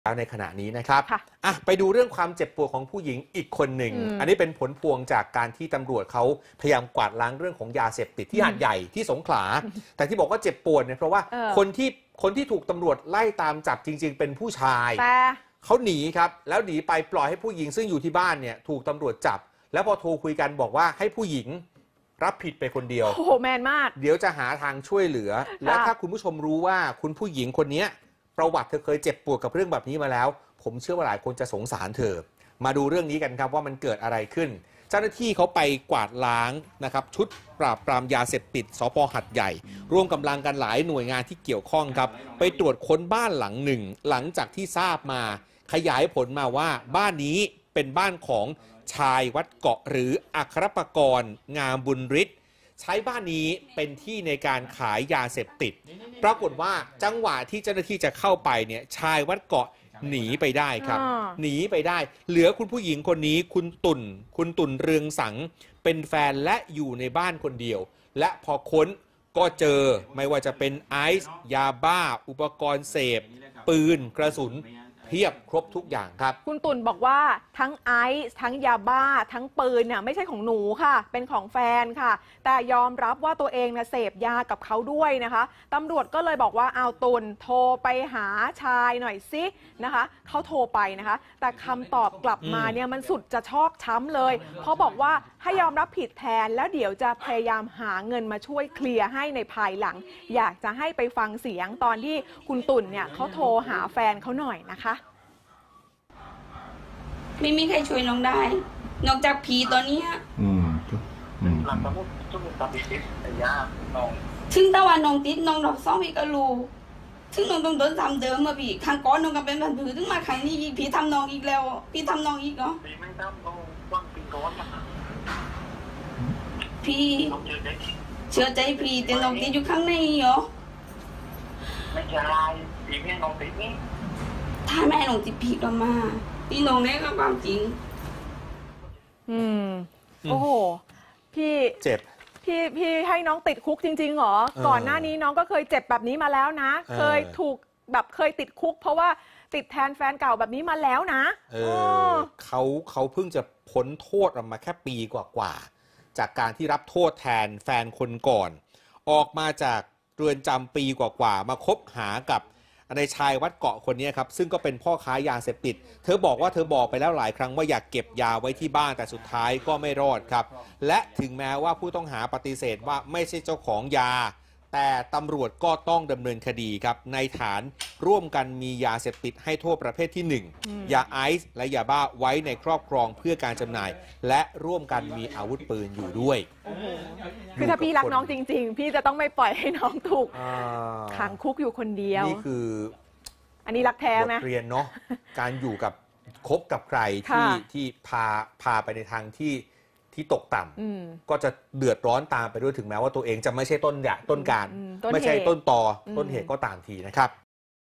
รักแท้ยอมติดคุกแทนแฟนค้ายาซ้ำเป็นรายที่ 2
สงขลา 10 พ.ค.-ตำรวจบุกจับ "ชายวัดเกาะ" เอเยนต์ค้ายาในหาดใหญ่ แต่ไหวตัวกระโดดหนีทัน ให้แฟนสาวที่อยู่ในบ้านรับผิดแทน ก่อนบอก "รักมาก" จะพยายามหาเงินมาช่วยเคลียร์ ขณะที่ฝ่ายหญิงเผยเคยรับเคราะห์ติดคุกแทนแฟนเรื่องค้ายาเป็นครั้งที่ 2

1.สารคดีเฉลิมพระเกียรติ
2.พระราชกรณียกิจ-พระมหากรุณาธิคุณ
3.คนไทยหัวใจ♥รักในหลวง